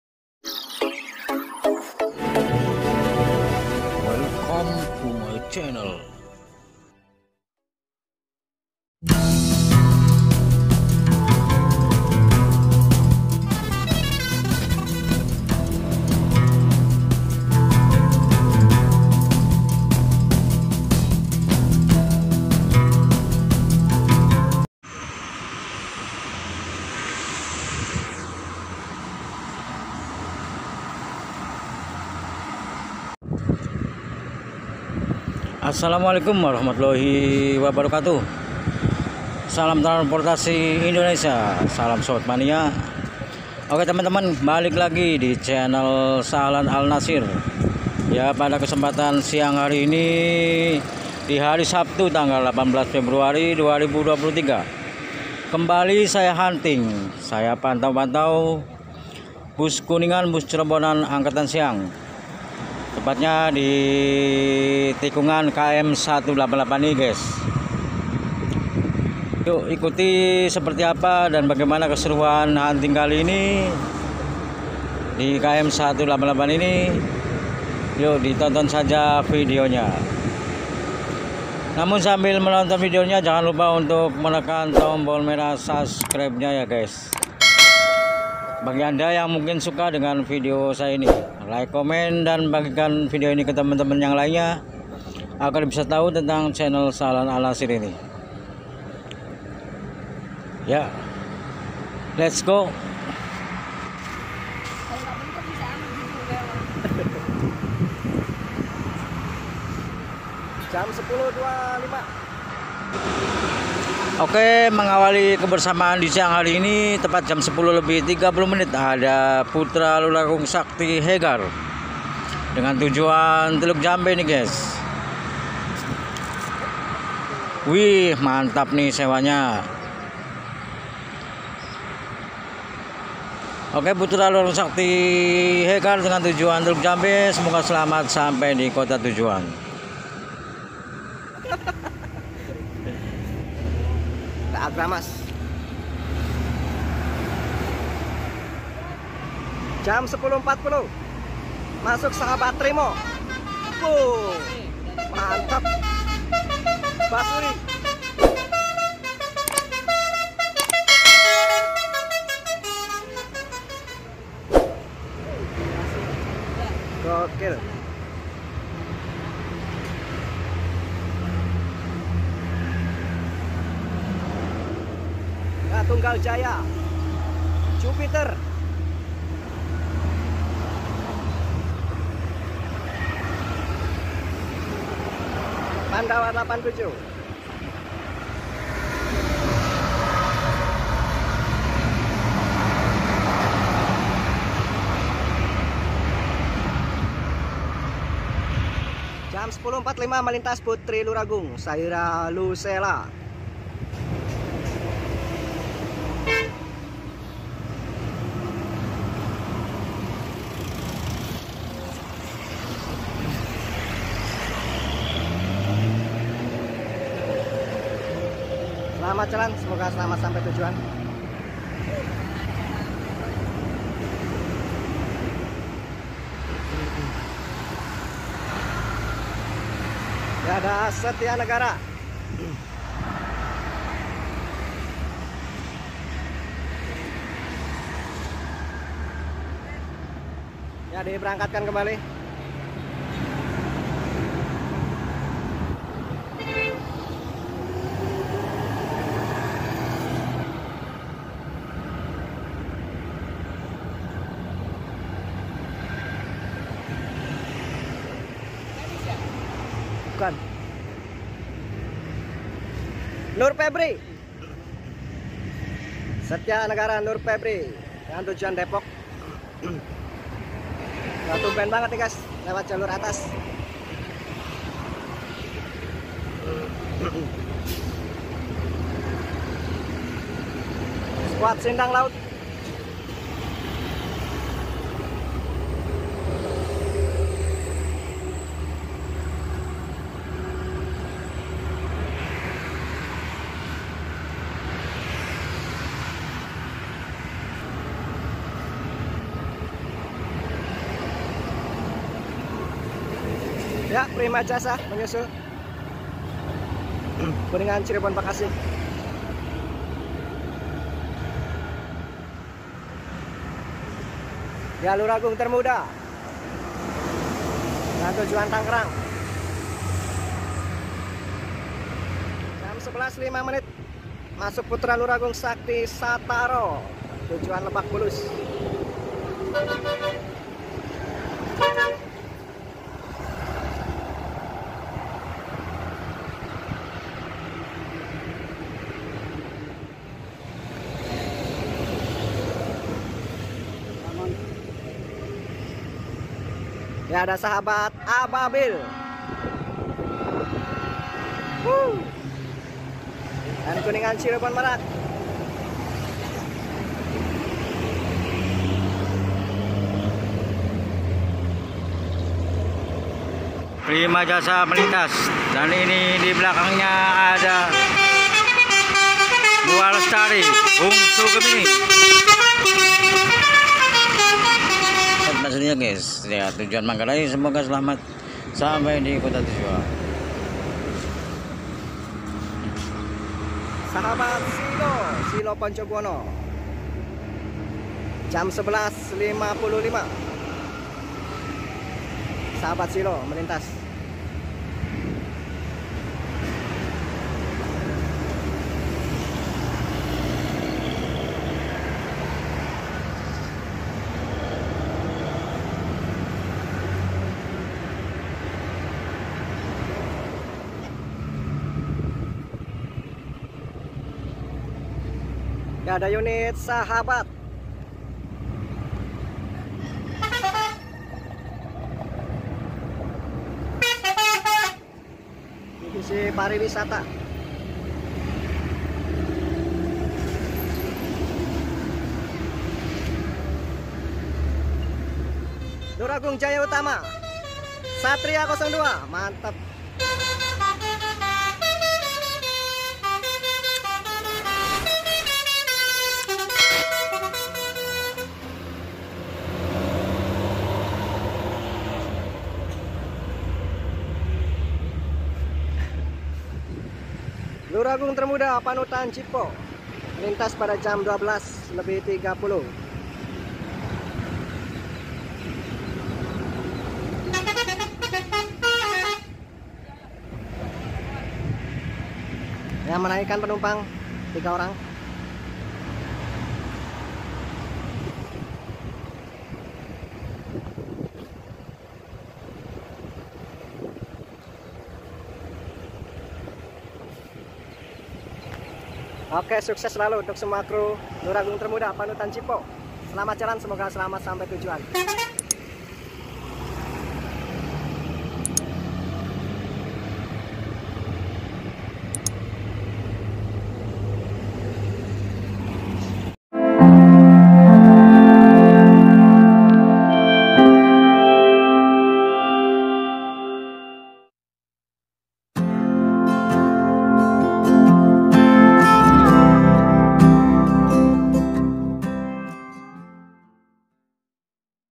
Tampil Garang‼️LJU "SATRIA 02" kini Pakai Basuri v3 ✅
Kembali Hunting & pantau Angkatan Siang Bus Kuningan, Bus Cirebonan di  tikungan Km. 188 menjelang pintu masuk  GT Palimanan.

Info terkini semakin Viralnya Telolet Basuri dan semakin banyak Armada Bus Kuningan dan Cirebonan yang memakainya...

seperti hari ini, yang Infonya launching perdana  " Satria 02 " dengan Telolet Basuri nya yang baru kemarin terpasang.

Seperti apa dan bagaimana keseruannya mangga di simak Videonya sampai habis hanya di :

Nyalain juga tanda  🔔  notifikasinya, agar Kamu jadi Orang pertama yang menonton, begitu Saya upload Video seru berikutnya